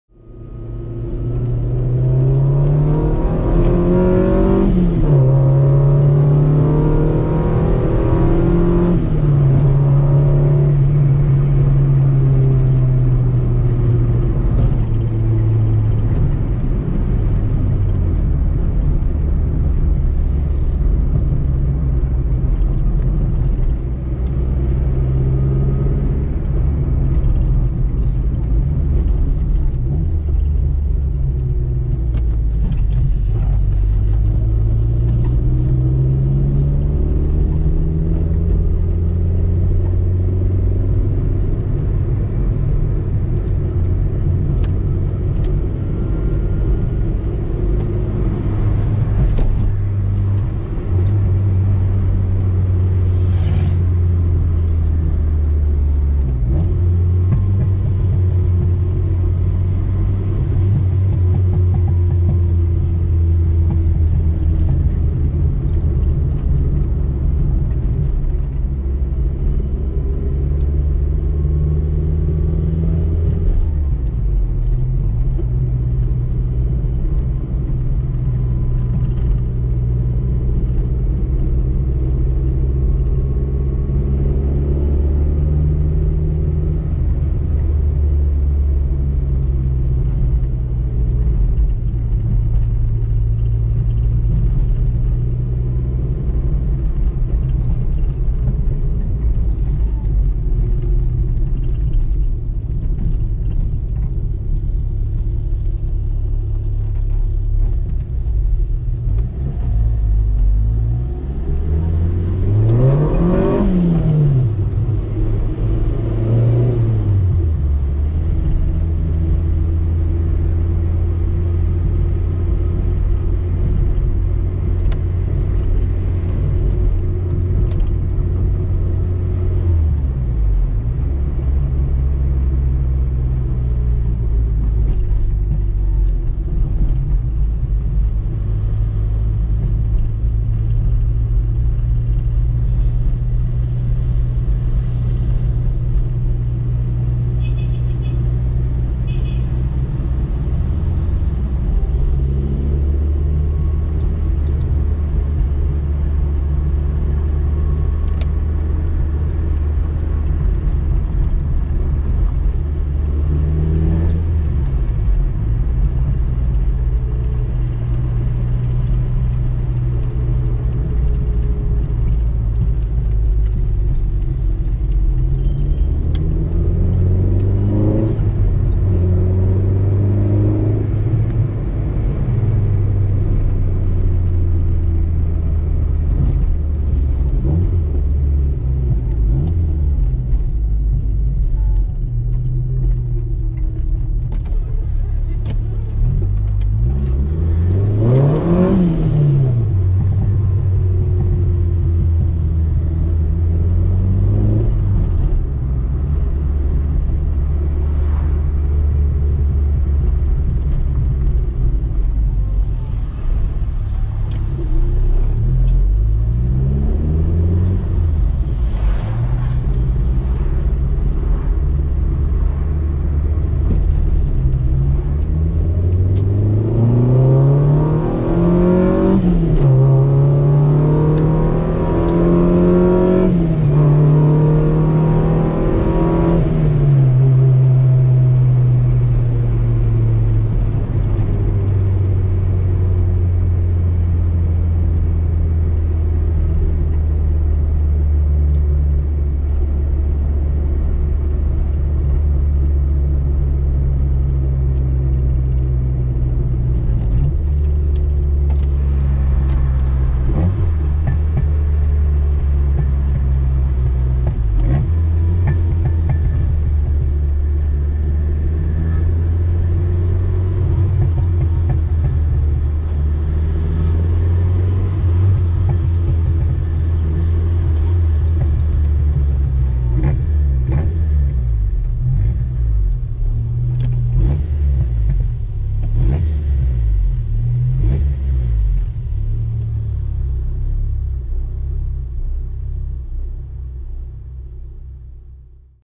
CORSA WIND 1.8 - POV | ROLÊ NOTURNO
Passeio de Corsa Wind 1.8 (swap) 1997 MPFI.
- Visão em 1ª pessoa | POV.